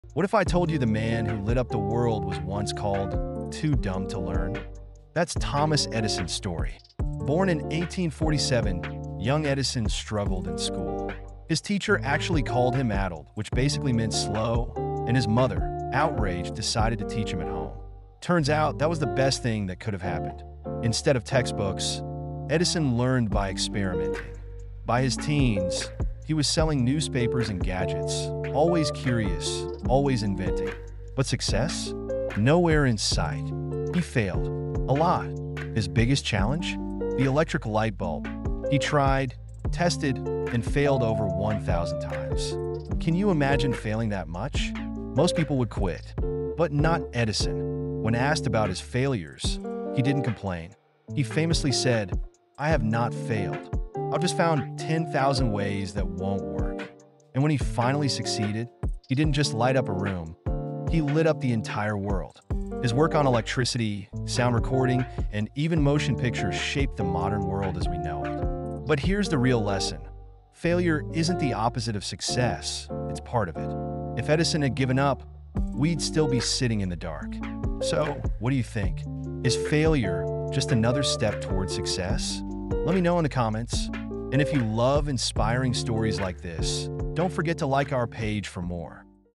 THOMAS EDISON
"Thomas Edison’s journey from school dropout to world-changing inventor wasn’t easy—he failed over 1,000 times before finally creating the light bulb! This is the inspiring story of how persistence and innovation can lead to greatness. Watch now and let us know—do you think failure is just a step toward success?"